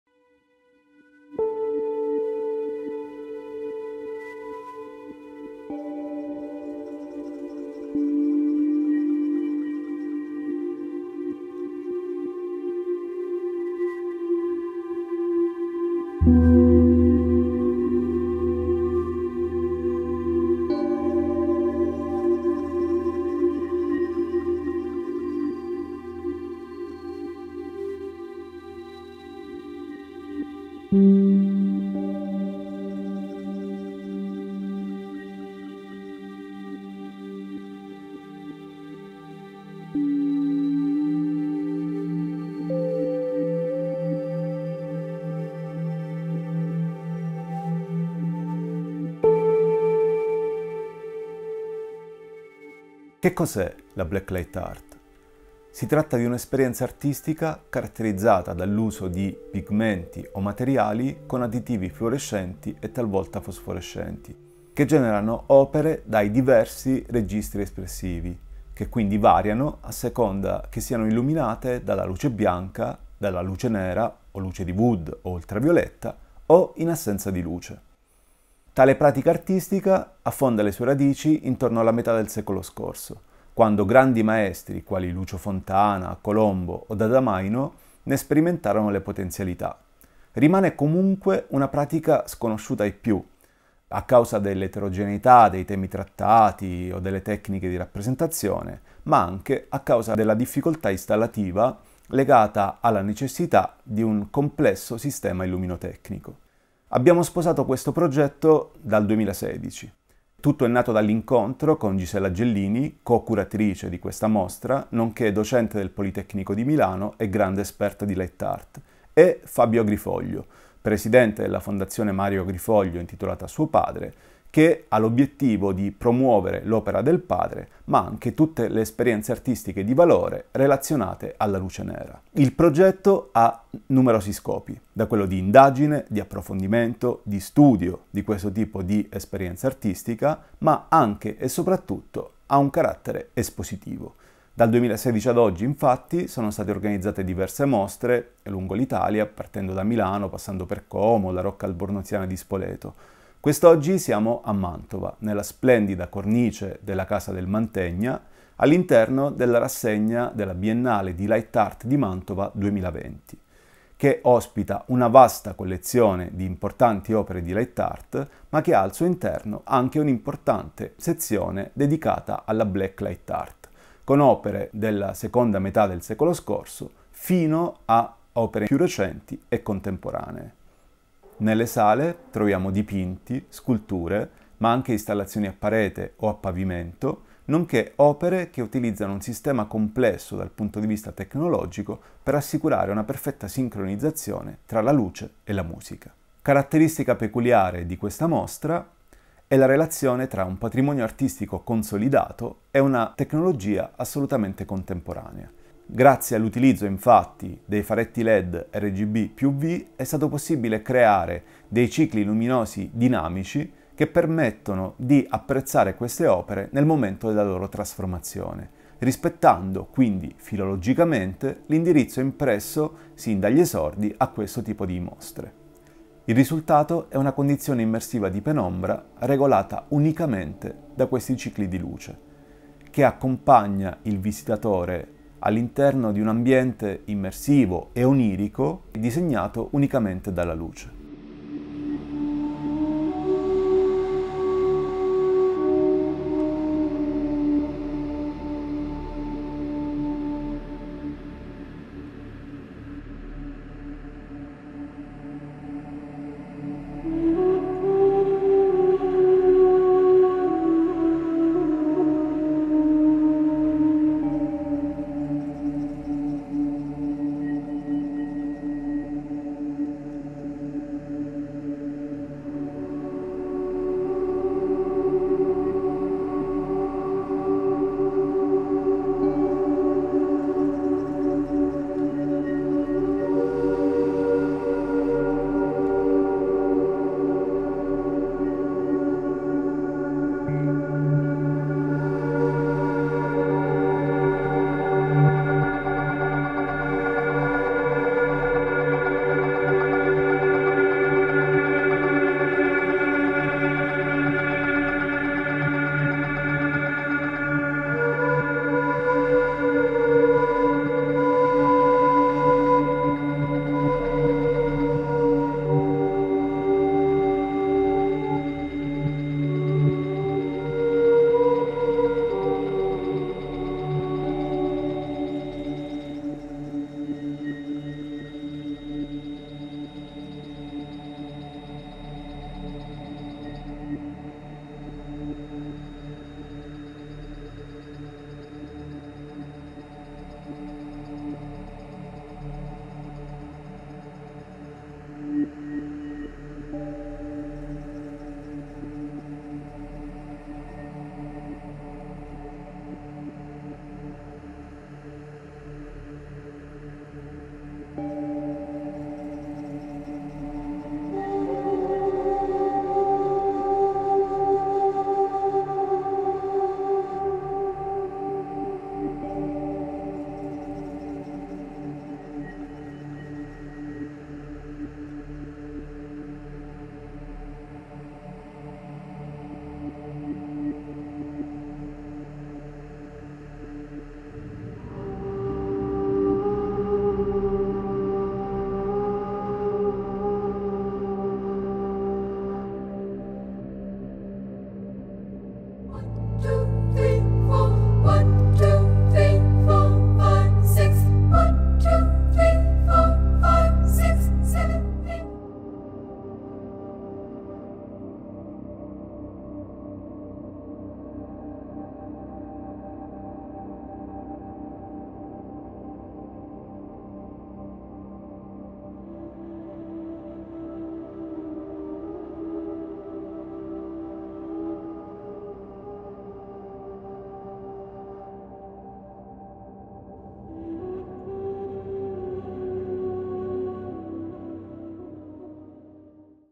Black Light Art alla Biennale di Mantova 2020
Videoracconto dell'esposizione di Black Light Art presso la Casa del Mantegna, curata da Gisella Gellini e Gaetano Corica, in collaborazione con Fabio Agrifoglio. La mostra è integrata nella Biennale di Light Art di Mantova 2020, curata da Vittorio Erlindo.
Video e contributo verbale del curatore della sezione di Black Light Art Gaetano Corica.